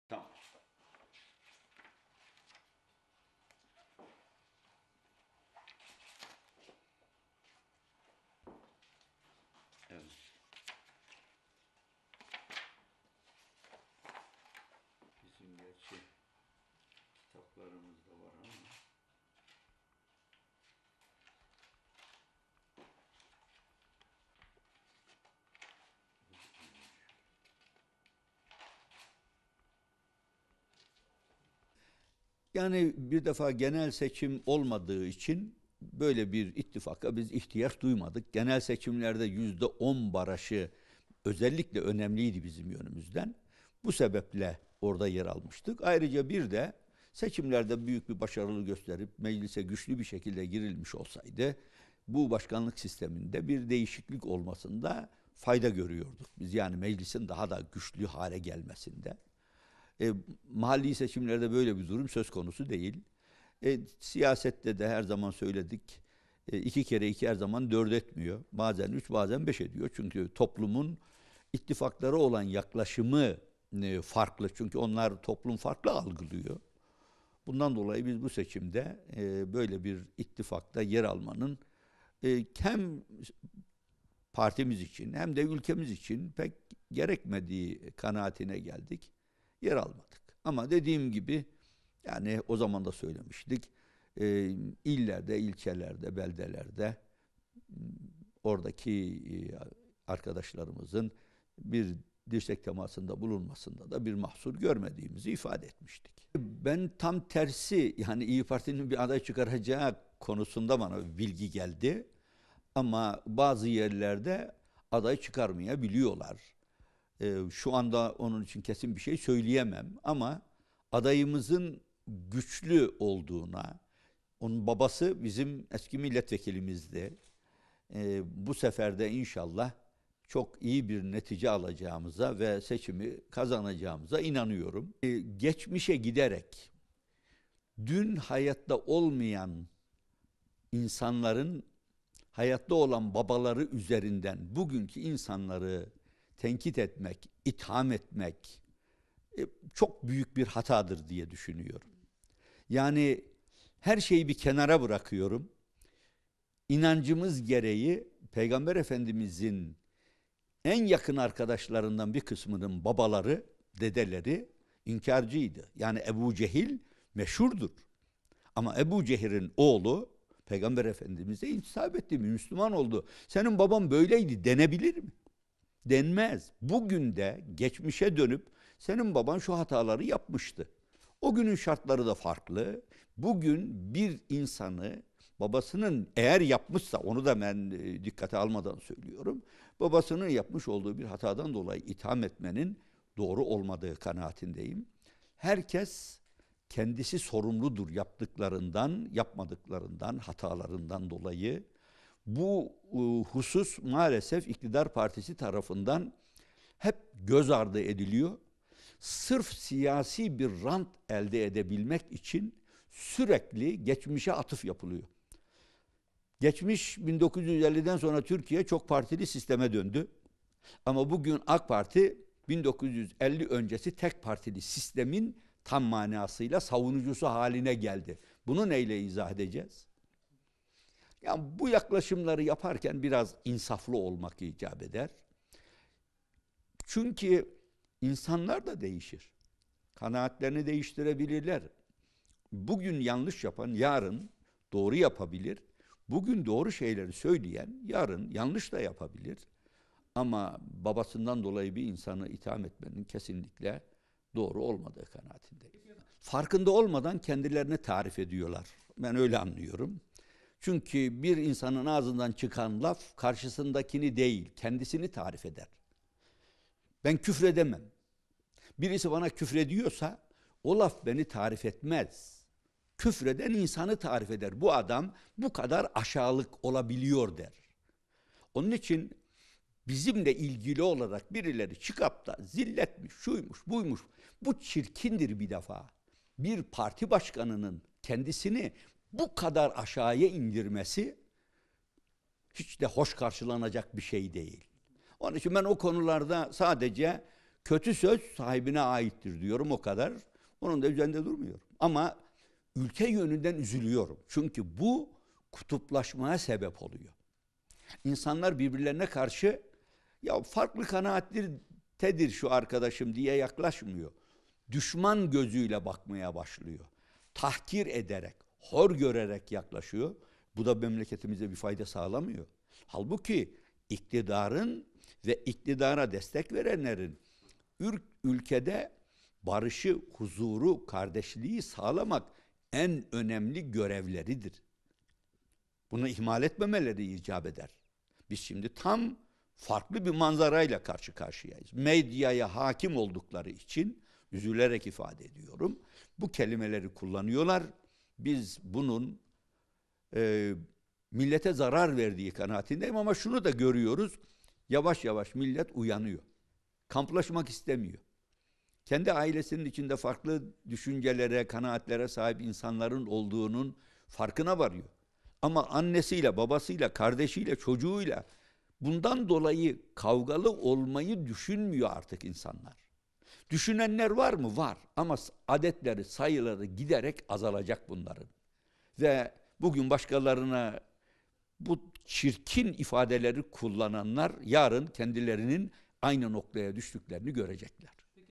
Karamollaoğlu: Geçmişe giderek insanları itham etmek hatadır
Saadet Partisi Genel Başkanı Temel Karamollaoğlu, CHP'nin İzmir Büyükşehir Belediye Başkan adayı Tunç Soyer'e, 12 Eylül döneminin askeri savcısı olan babası üzerinden yöneltilen eleştirilerin doğru olmadığını söyledi. Karamollaoğlu, "Geçmişe giderek hayatta olmayan insanlar üzerinden bugünkü insanları tenkit etmek, itham etmek çok büyük bir hatadır" dedi.

SP Genel Başkanı Temel Karamollaoğlu, DHA'ya yaptığı açıklamada, 24 Haziran genel seçimlerinde yüzde 10 barajının önemli olduğunu, bu nedenle ittifaka girdiklerini; ancak yerel seçimlerde ittifakta yer almaya gerek duymadıklarını söyledi. Karamollaoğlu, "Siyasette her zaman söyledik, 'iki kere iki dört etmiyor, bazen üç, bazen beş ediyor. Çünkü toplumun ittifaklara olan yaklaşımı farklı. Çünkü toplum farklı algılıyor. Bundan dolayı biz bu seçimde böyle bir ittifakta yer almanın hem partimiz, hem ülkemiz için pek gerekmediği kanaatine vardık ve yer almadık. Ama dediğim gibi illerde, ilçelerde, oradaki arkadaşlarımızın dirsek temasında bulunmasında bir mahsur görmüyoruz" diye konuştu.